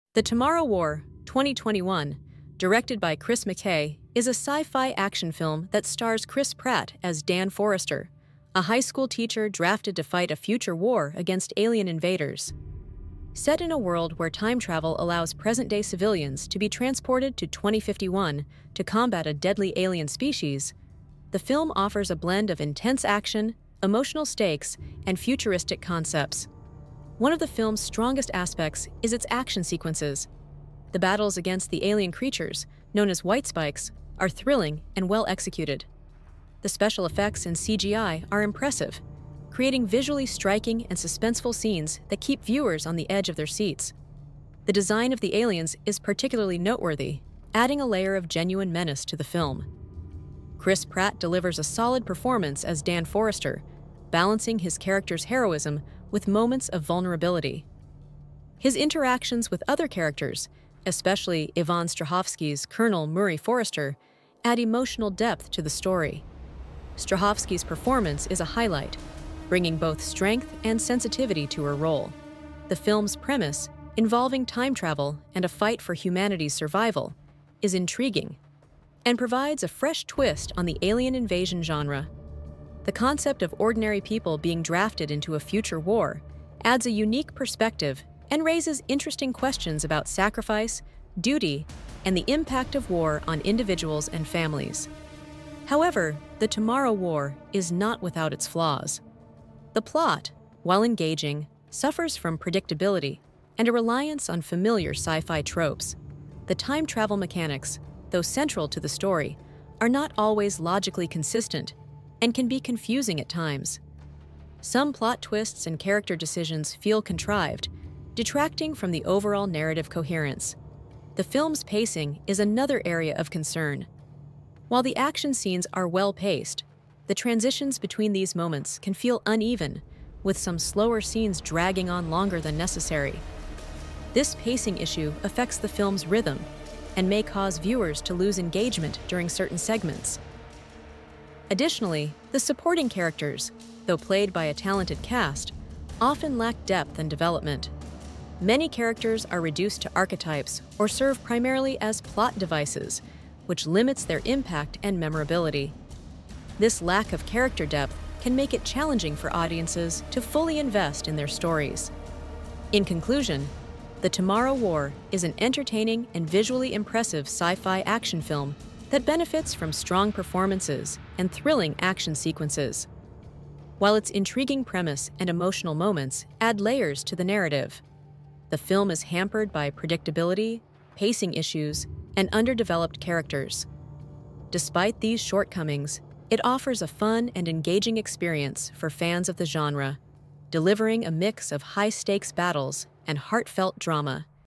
FILM Review: The Tomorrow war
"The Tomorrow War" (2021), directed by Chris McKay, is a sci-fi action thriller starring Chris Pratt as Dan Forester, a high school teacher drafted to fight a future war against a deadly alien species. Transported to 2051 through time travel, Dan joins a global force of soldiers and civilians to combat the alien threat and save humanity. As the stakes rise, he confronts personal challenges and uncovers secrets about the invasion. With intense action, emotional depth, and stunning visual effects, "The Tomorrow War" is a gripping battle for the future of mankind.